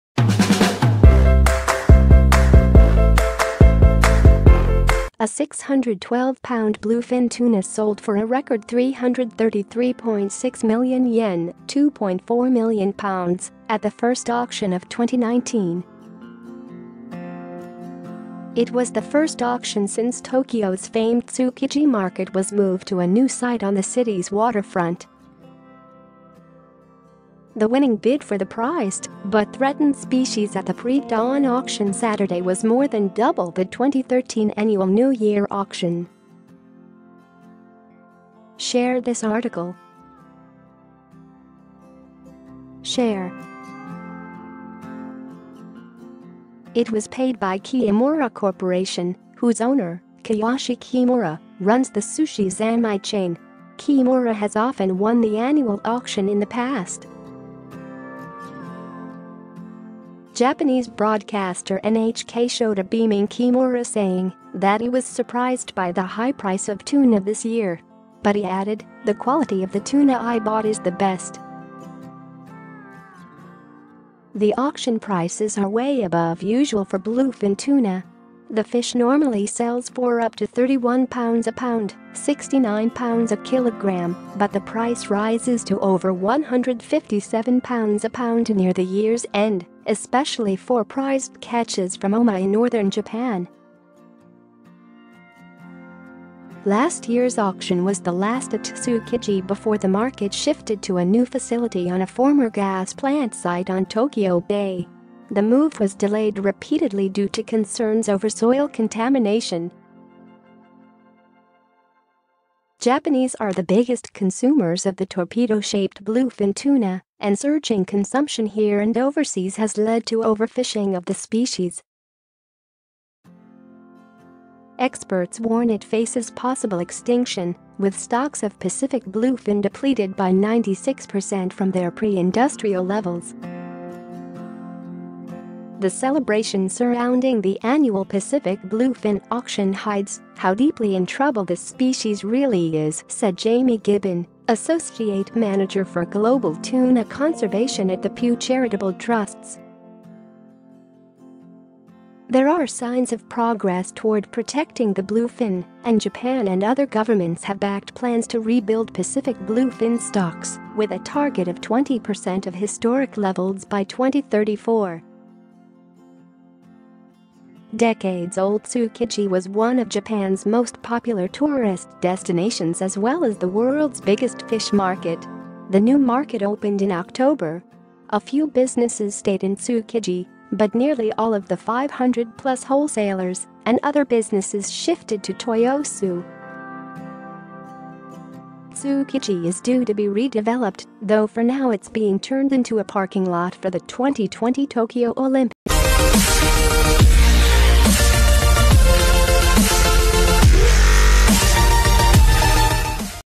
A 612lb Bluefin Tuna sells for record £2.4MILLION in Tokyo fish market - Daily News
A,612lb,Bluefin,Tuna,sells,record,2,4MILLION,Tokyo,fish,market A 612-pound bluefin tuna sold for a record 333.6 million yen (£2.4m) at the first auction of 2019.
It was the first auction since Tokyo's famed Tsukiji market was moved to a new site on the city's waterfront.
The winning bid for the prized but threatened species at the predawn auction Saturday was more than double the 2013 annual New Year auction.
It was paid by Kiyomura Corp., whose owner, Kiyoshi Kimura, runs the Sushi Zanmai chain. Kimura has often won the annual auction in the past.
Japanese broadcaster NHK showed a beaming Kimura saying that he was surprised by the high price of tuna this year. But he added, 'The quality of the tuna I bought is the best.'
The auction prices are way above usual for bluefin tuna. The fish normally sells for up to £31 a pound (£69 a kilogram), but the price rises to over £157 a pound near the year's end, especially for prized catches from Oma in northern Japan.
Last year's auction was the last at Tsukiji before the market shifted to a new facility on a former gas plant site on Tokyo Bay. The move was delayed repeatedly due to concerns over soil contamination.
Japanese are the biggest consumers of the torpedo-shaped bluefin tuna, and surging consumption here and overseas has led to overfishing of the species.
Experts warn it faces possible extinction, with stocks of Pacific bluefin depleted by 96 percent from their pre-industrial levels.
'The celebration surrounding the annual Pacific bluefin auction hides how deeply in trouble this species really is,' said Jamie Gibbon, associate manager for global tuna conservation at The Pew Charitable Trusts.
There are signs of progress toward protecting the bluefin, and Japan and other governments have backed plans to rebuild Pacific bluefin stocks, with a target of 20 percent of historic levels by 2034.
Decades-old Tsukiji was one of Japan's most popular tourist destinations as well as the world's biggest fish market. The new market opened in October. A few businesses stayed in Tsukiji, but nearly all of the 500-plus wholesalers and other businesses shifted to Toyosu.
Tsukiji is due to be redeveloped, though for now it's being turned into a parking lot for the 2020 Tokyo Olympics.